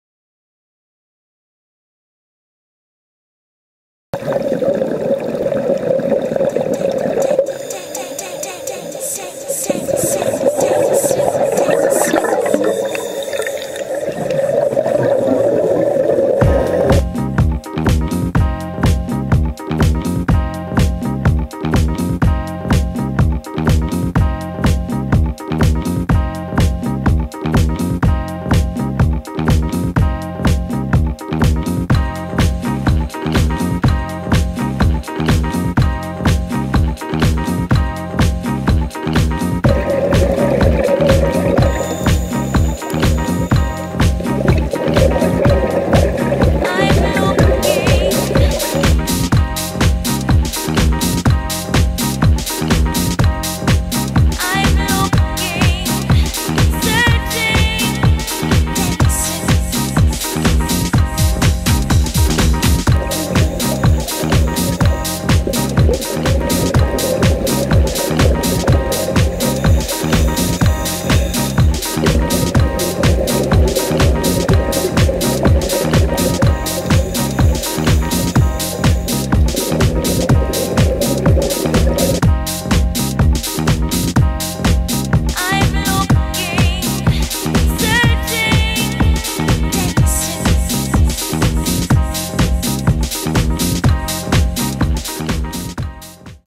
Scuba - Roatan, Honduras 2008
m-dot and miguelitingo learn to scuba dive in Roatan